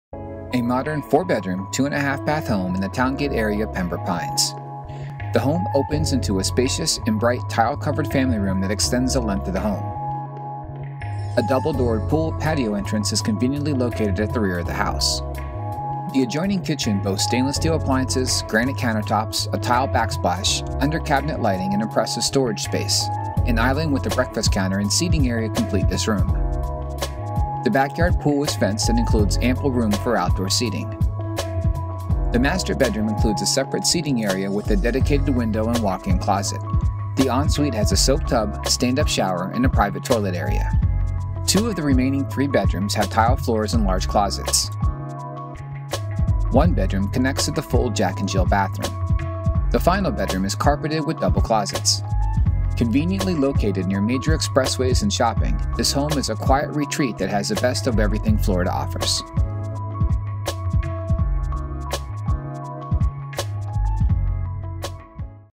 Gorgeous 4 bedroom 2.5 bath home in Pembroke Pines
Gorgeous 4 bedroom, 2.5 bath home with an open floor plan and beautiful custom tile floors. Spacious master bedroom with sitting area. Master bath features Roman tub and separate step in shower. Beautiful upgraded kitchen with granite counter tops. Backyard features a beautiful solar heated pool with patio. This desirable neighborhood has excellent "A" rated schools and community park with basketball courts, tennis courts, soccer field, baseball diamond and large pool.